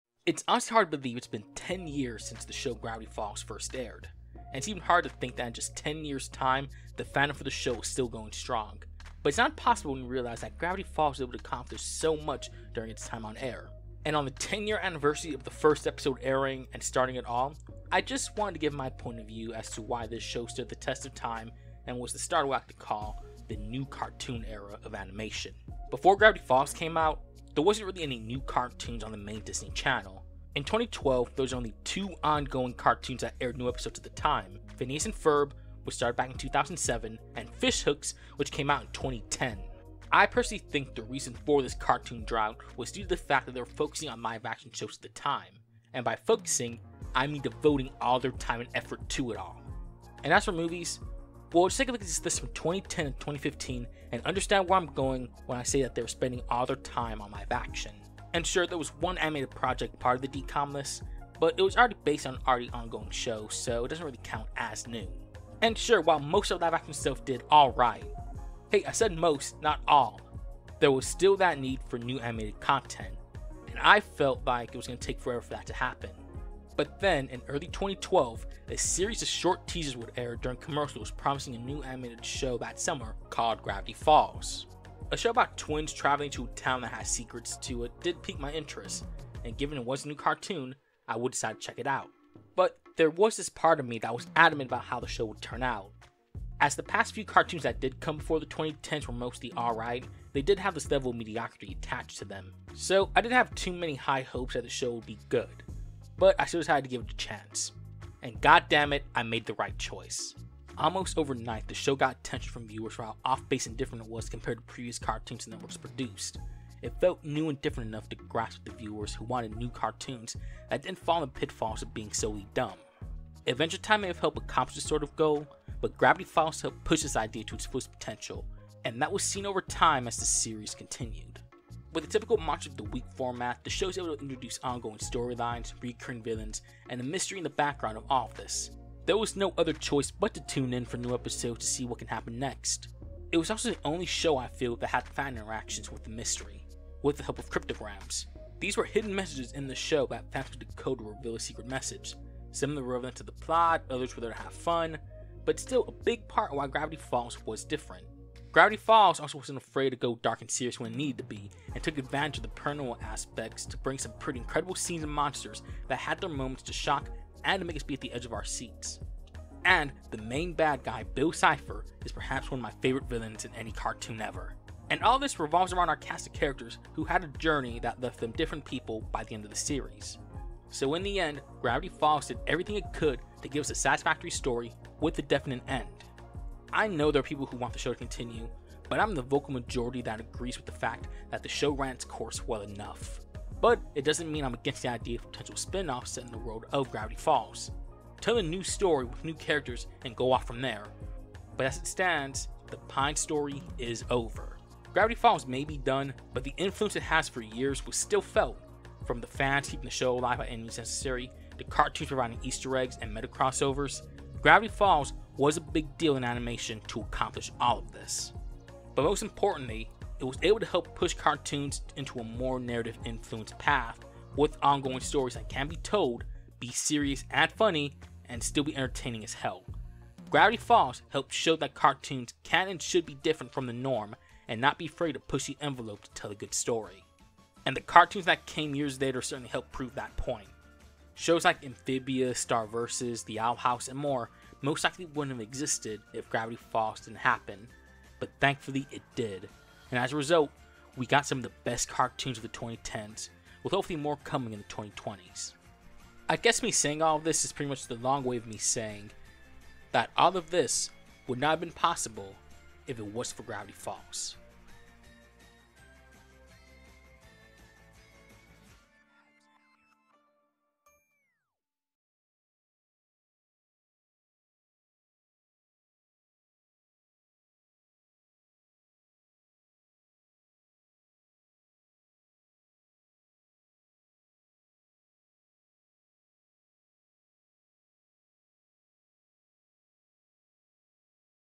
Why Gravity Falls Was a BIG DEAL For Animation
Today for the 10 Year Anniversary of Gravity Falls First Episode, Let Me Talk about Why This Show Was a Big Deal for Animation as a Whole!